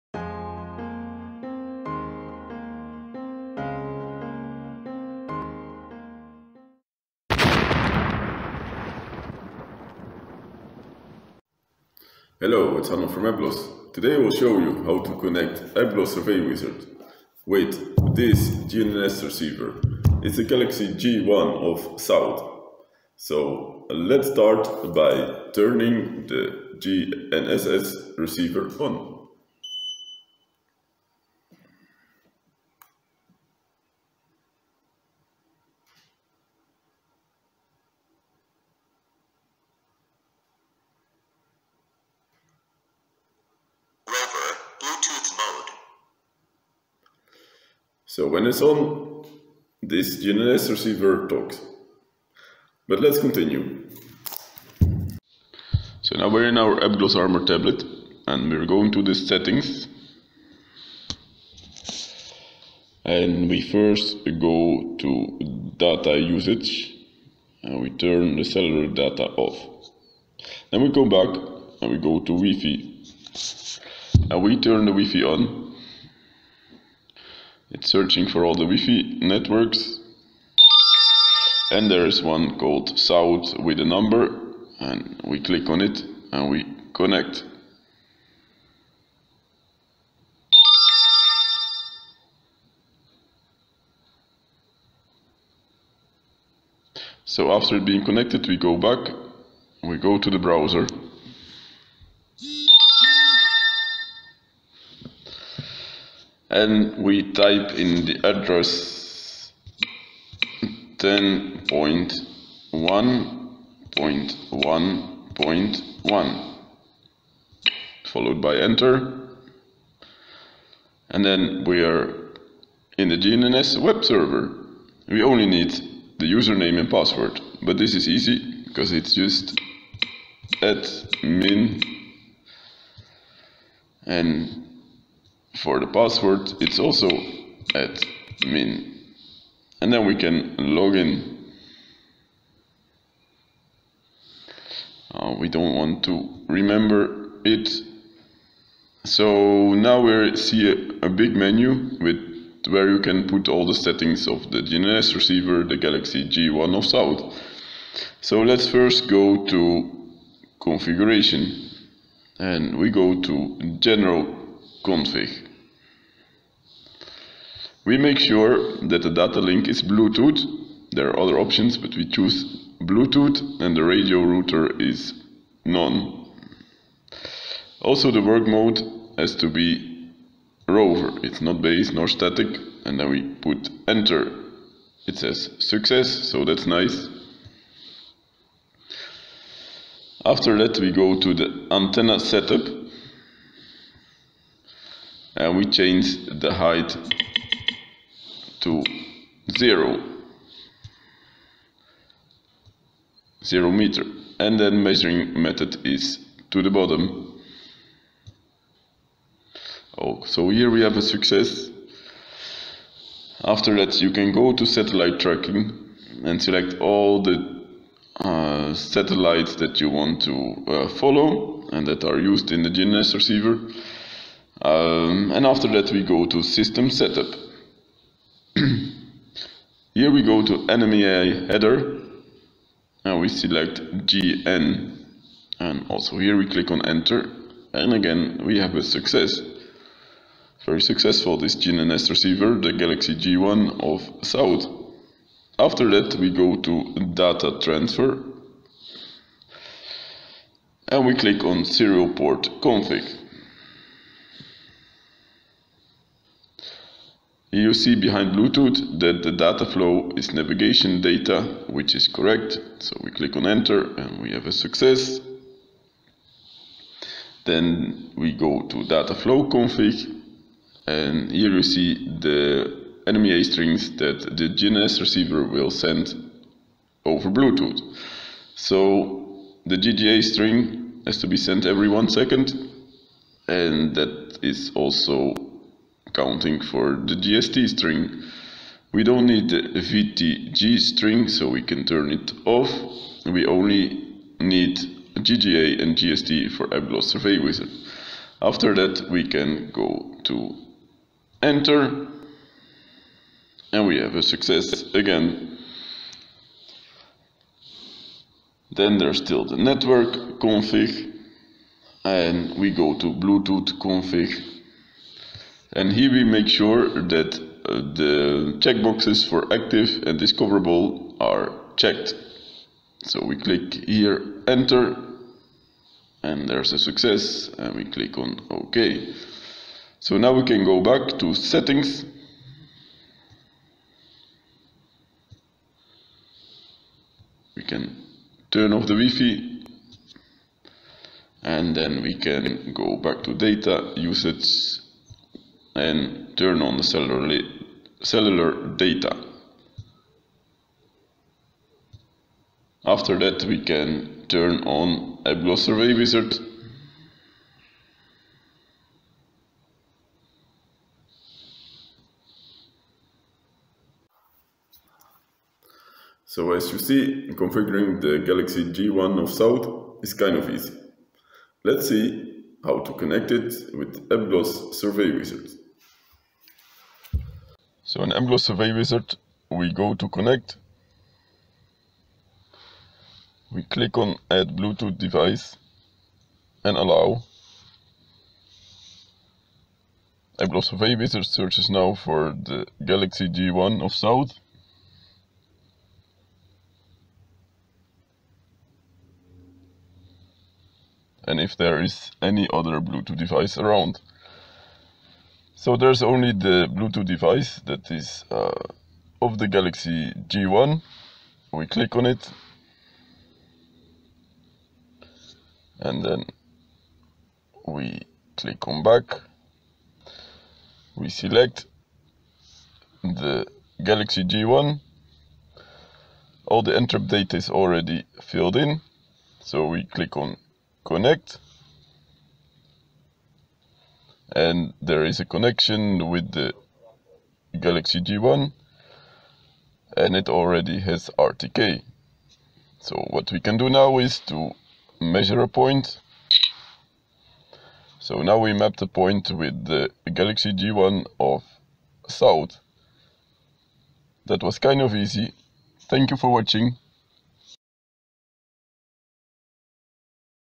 Configuring Galaxy G1 GNSS receiver of South without Egstar for Apglos Survey Wizard
Here you learn how to set up the South Galaxy G1 GNSS receiver without Egstar so you can use this GNSS receiver with Apglos Survey Wizard.